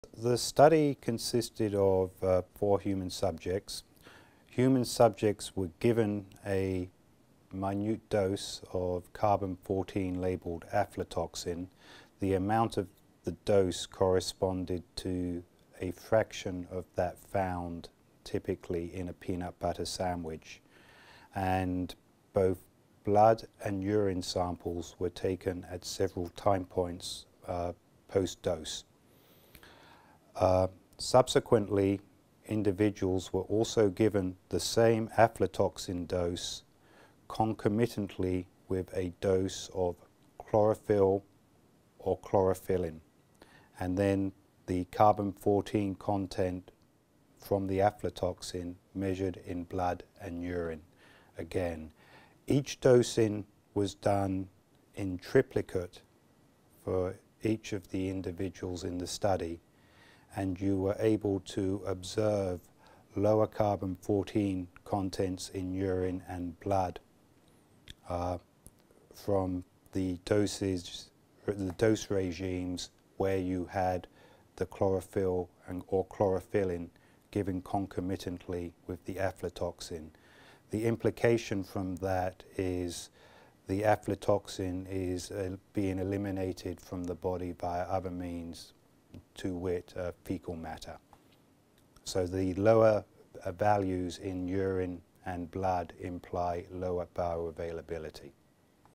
Preventing toxic effects of peanut butter
LLNL researchers Graham Bench (on this video clip) and Ken Turteltaub found that giving someone a small dose of chlorophyll or chlorophyllin — found in green leafy vegetables such as spinach, broccoli and kale — could reverse the effects of aflatoxin poisoning. Aflatoxin is a potent, naturally occurring carcinogenic mycotoxin that is associated with the growth of two types of mold: Aspergillus flavus and Aspergillus parasiticus. Food and food crops most prone to aflatoxin contamination include corn and corn products, cottonseed, peanuts and peanut products, tree nuts and milk.